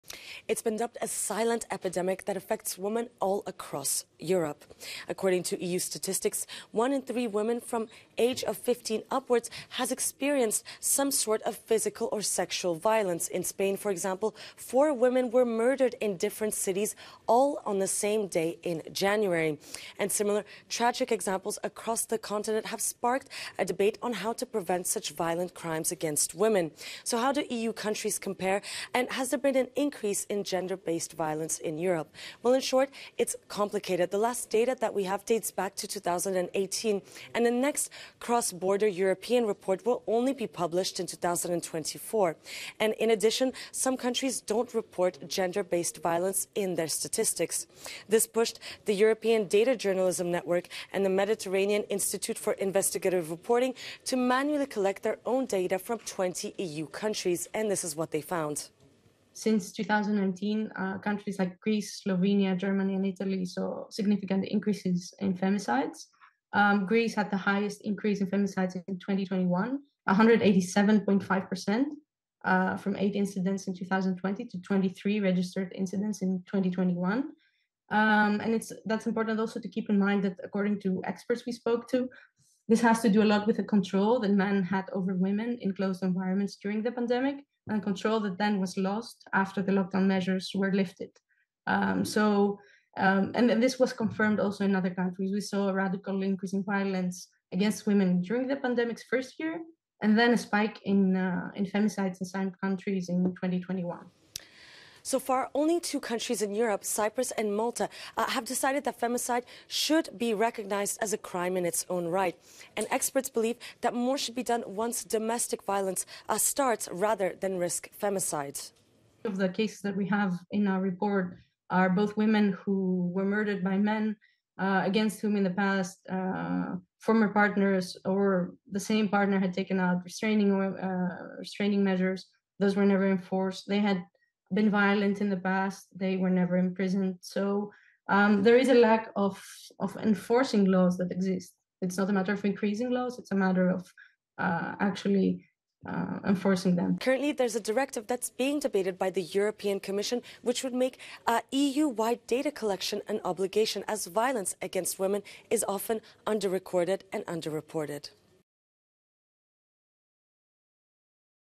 L'Europe n'arrive pas à trouver la solution pour mettre fin aux féminicides
En cette journée du 8 mars, TheCube fait le point sur les féminicides à travers l'Europe.